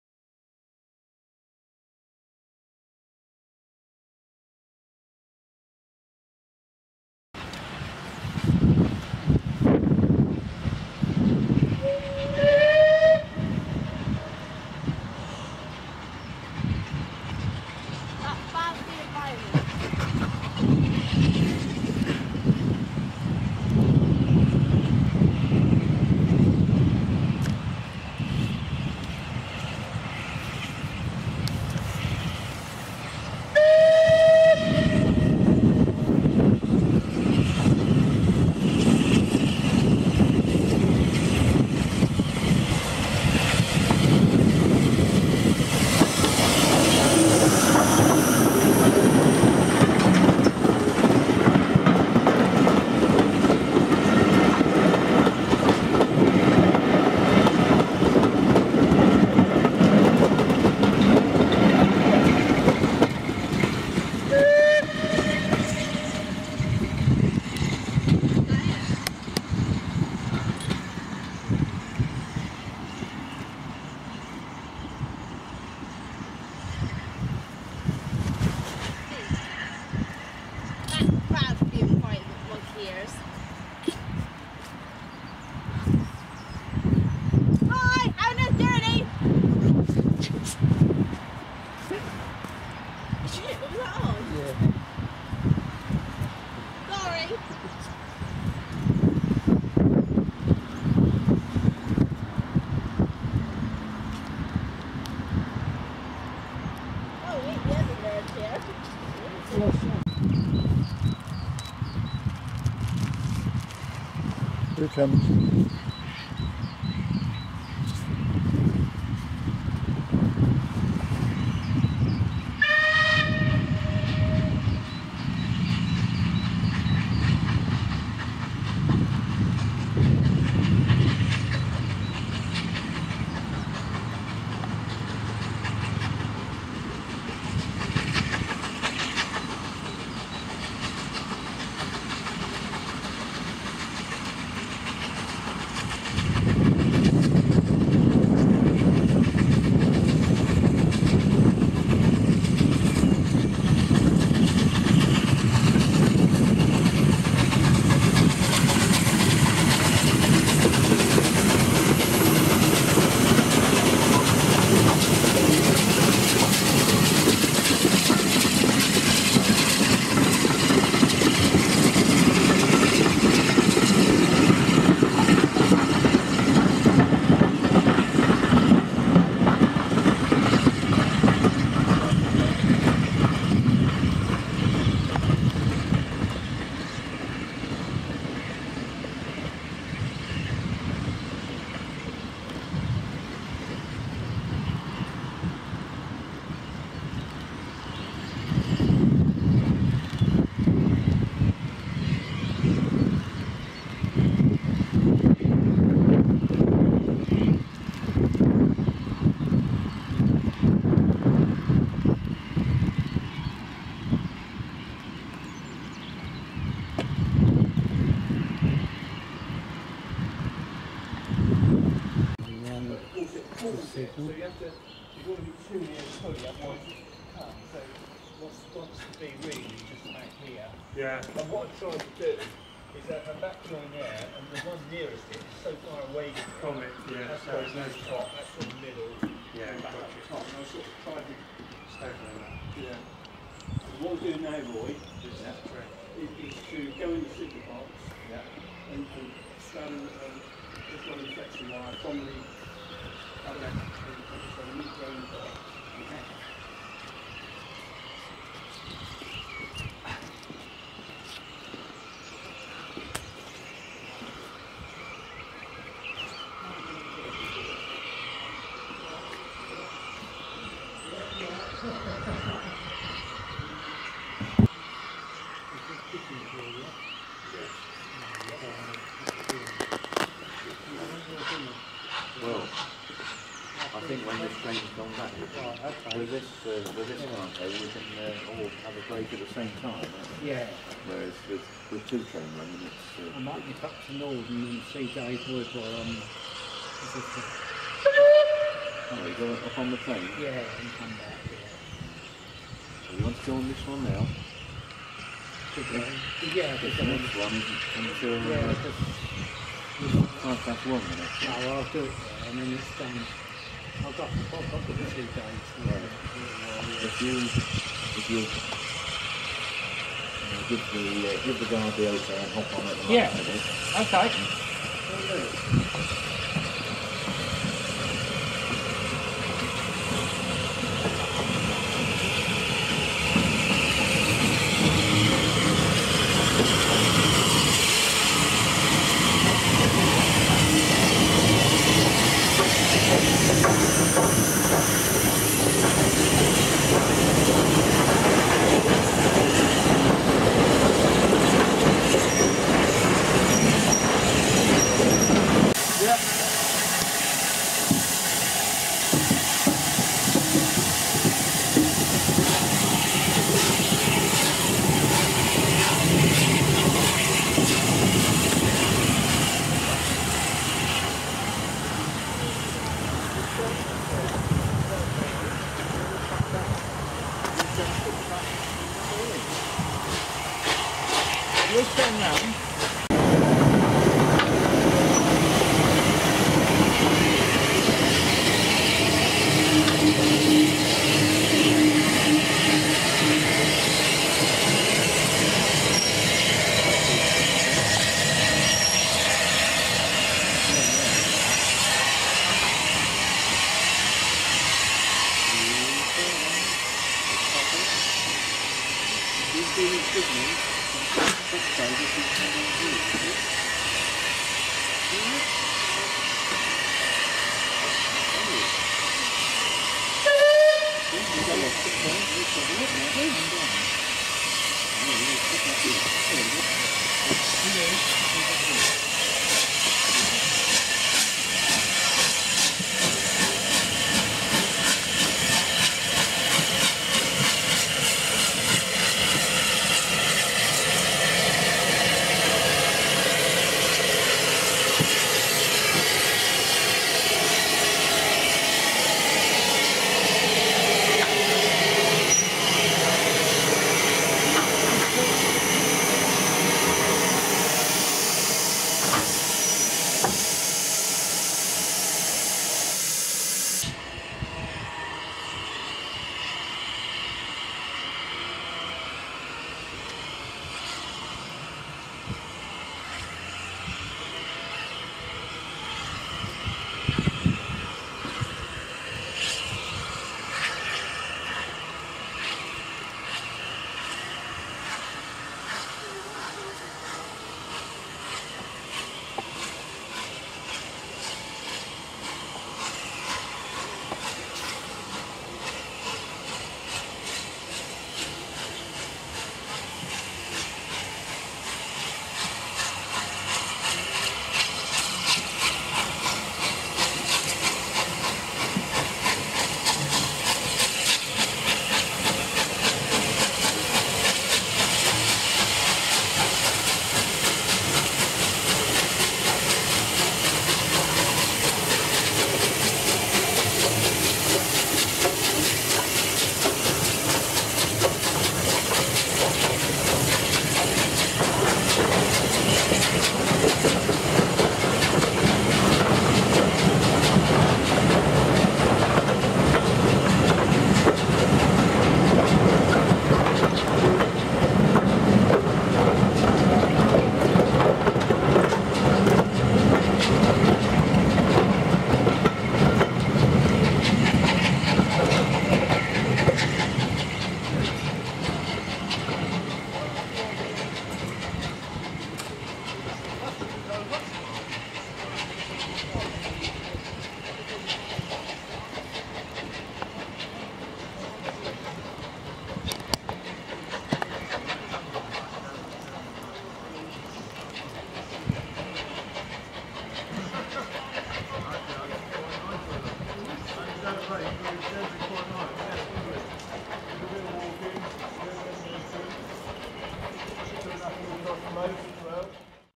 Swanage Railway Blue Service 23rd Februaury Part 1
On a dull wet windy horrible day, my 2011 season has started at Swanage Railway, on the Blue Service during the Febraury half term 2011. Battle of Britain class 34070 Manston running all day. HD videos is becoming a nightmare to upload to Youtube due to crap connection so i am thinking ways to upload them